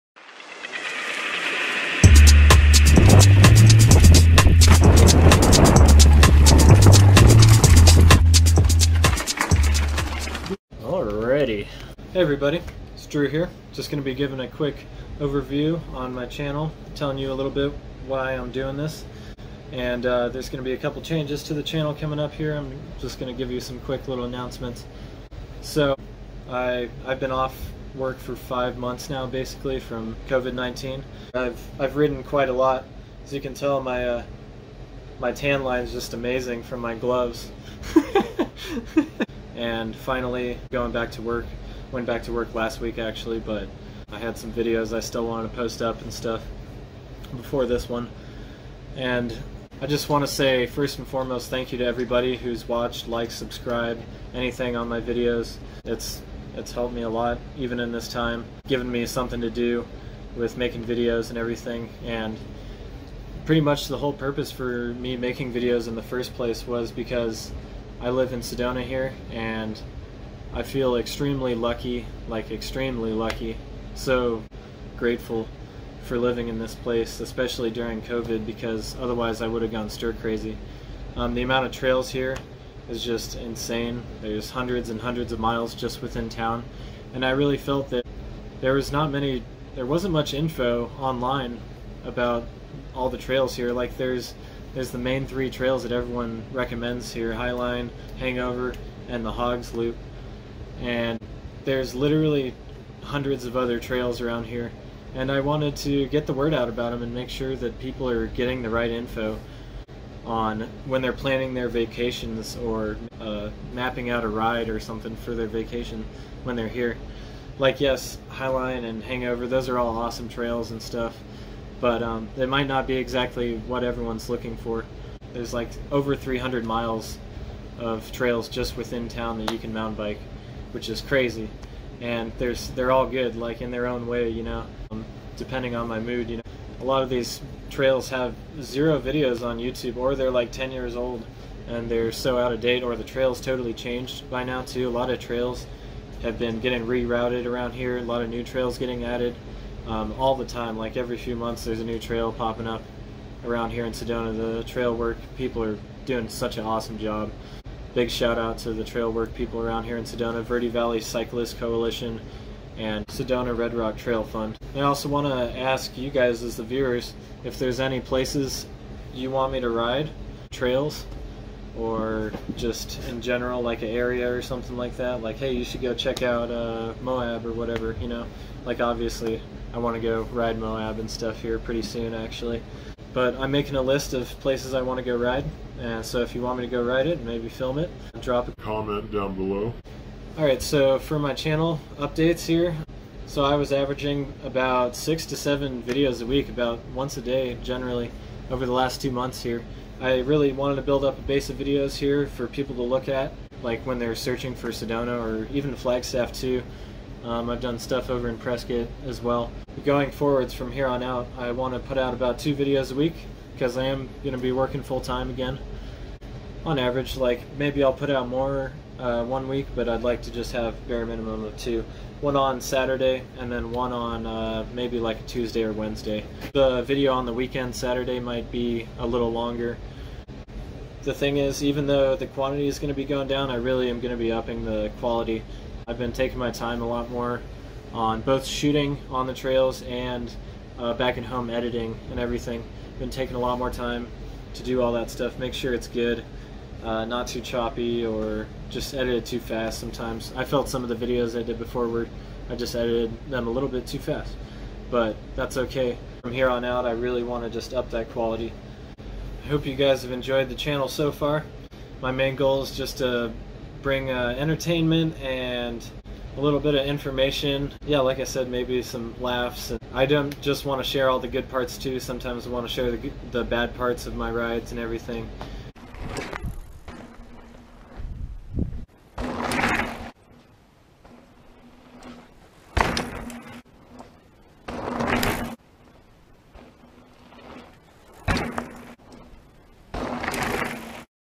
Channel Updates!
Updating you on changes that are happening with my channel, like my new intro, as well as give background on why I even started doing this in the first place.